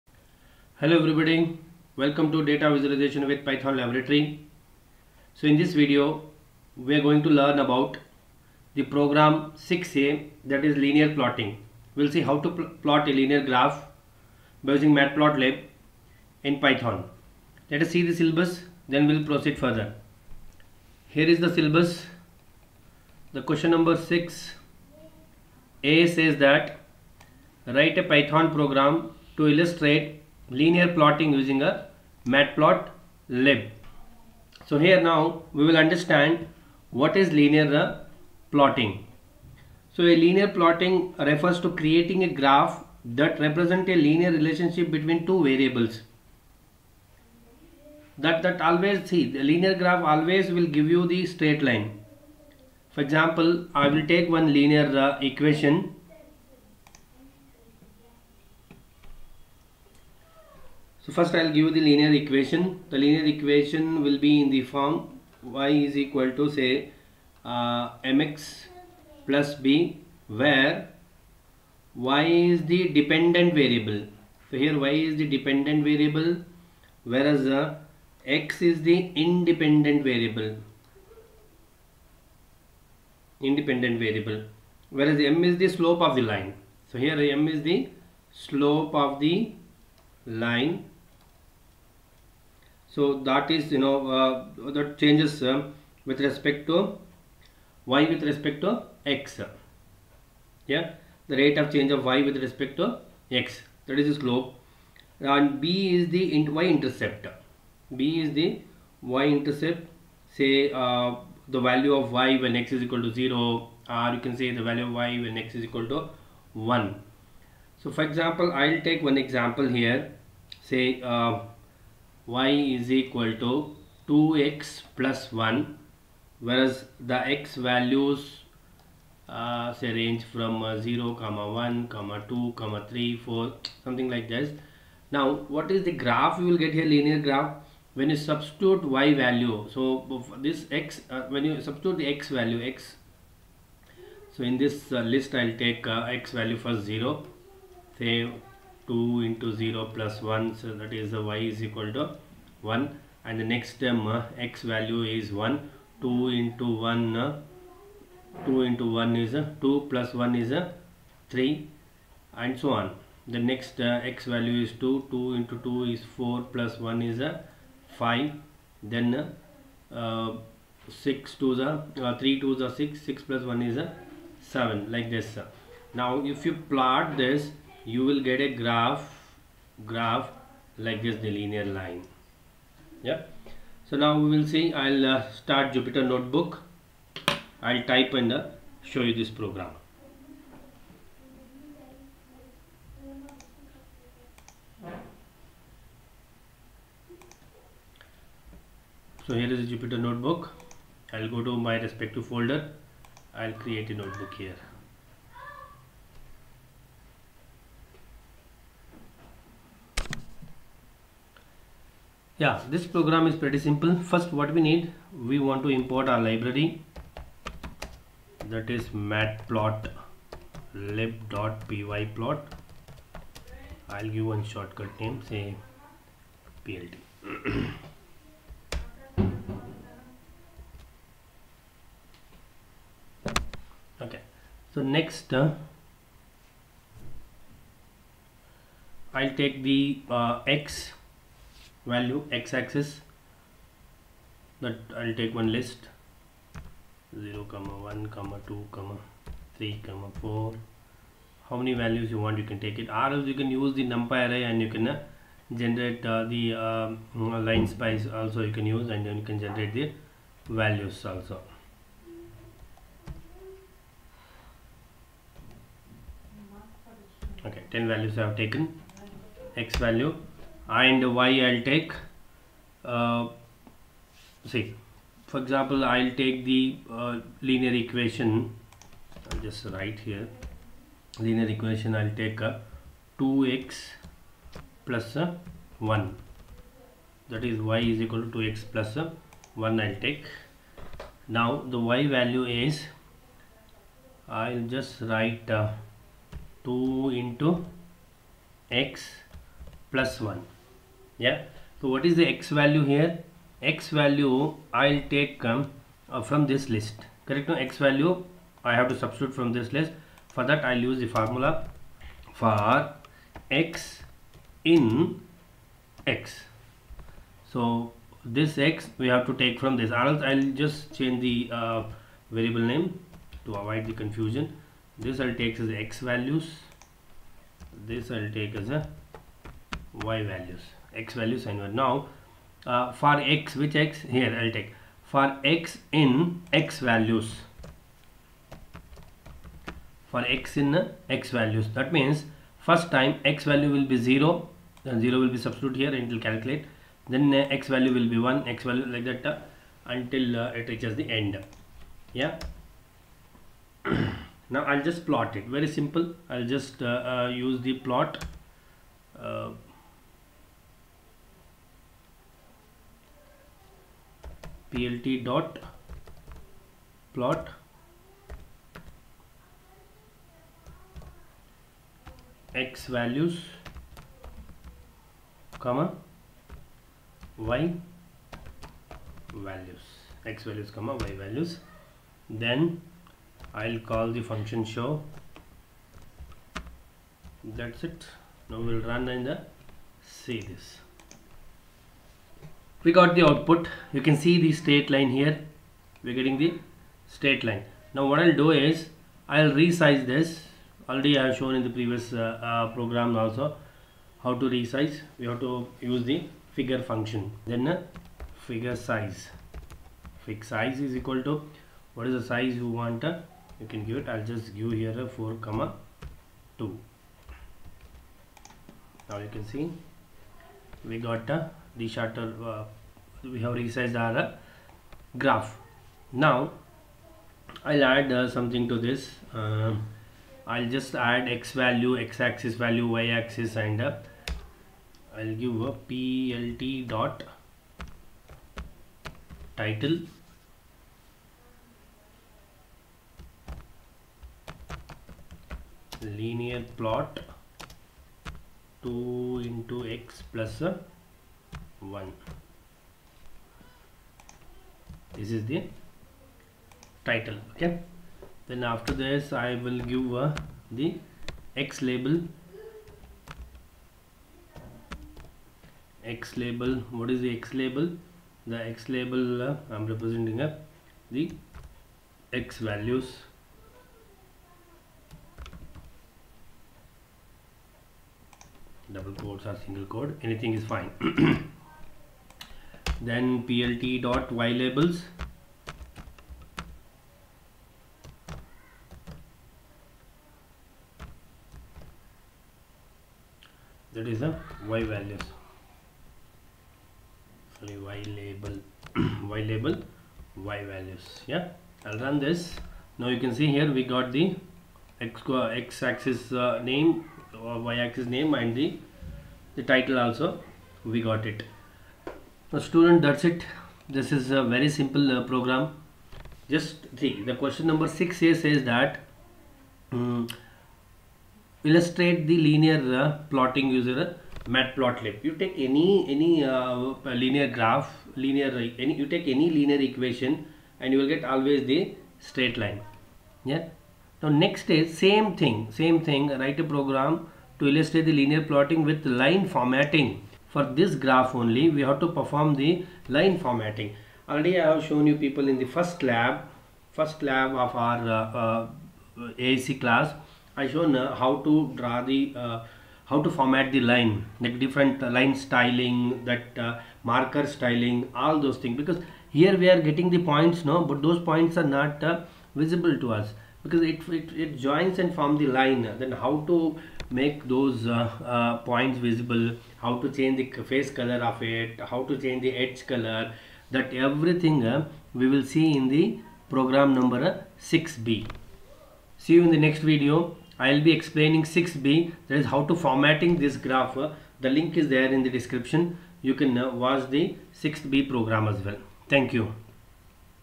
6a Linear Plotting using Matplotlib | Data Visualization with Python | VTU 3rd Semester AEC Lab
6a. Write a Python program to illustrate Linear Plotting using Matplotlib.

Data Visualization with Python (BCS358D)  Ability Enhancement Course for 3rd semester

VTU 2022 Scheme Laboratory

IK Tutorial Bangalore